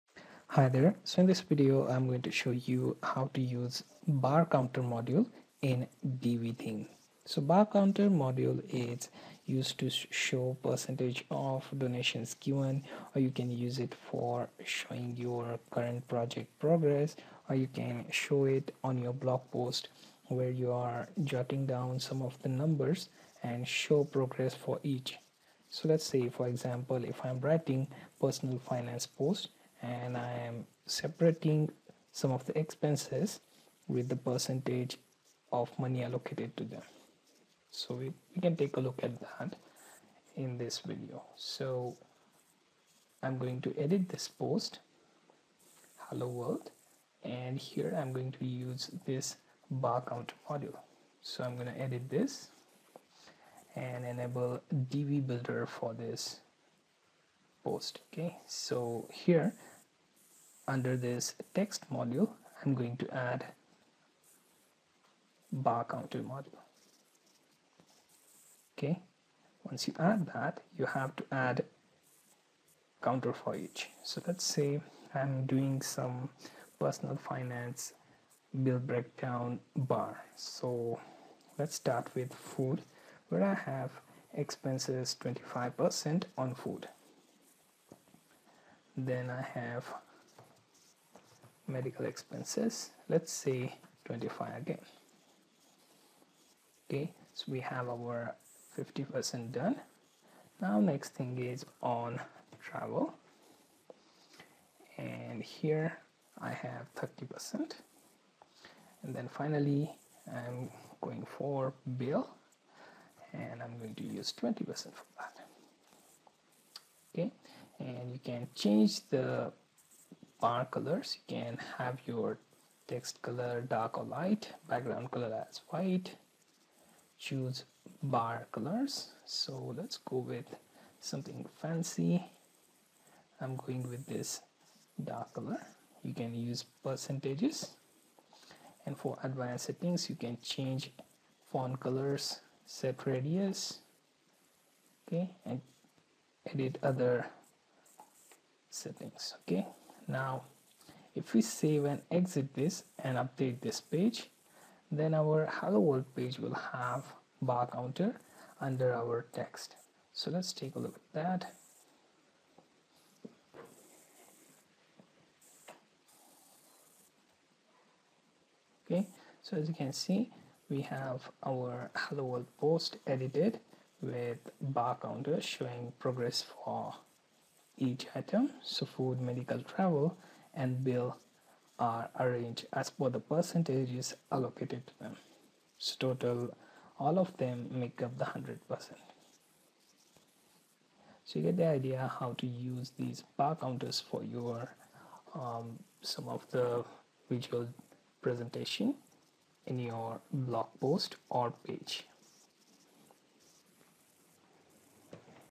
Divi Bar Counter Module in WordPress
Learn how to Add Bar Counter in WordPress using Divi theme.